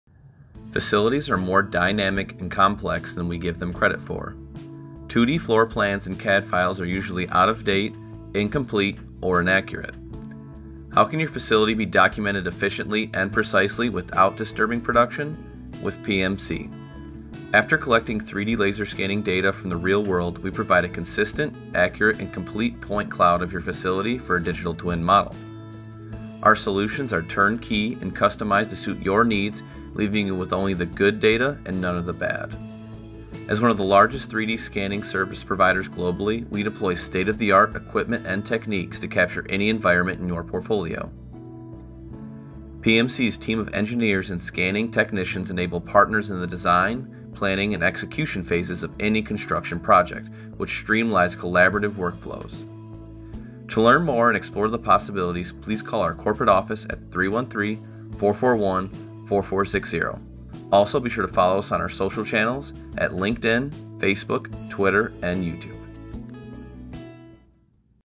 Know Your Facility | 3D Laser Scanning for Production Facilities
Production Facilities are some of the most complex & dynamic structures humans have created. The documentation of your facility should hold the details for every inch of production. PMC has the technology and experience to provide the most in depth 3D Laser Scanning with no disruption to production. PMC Digital Twins & Laser Scanning provides the detailed information to streamline your facility.

PMC, Your Partner In Productivity Improvement!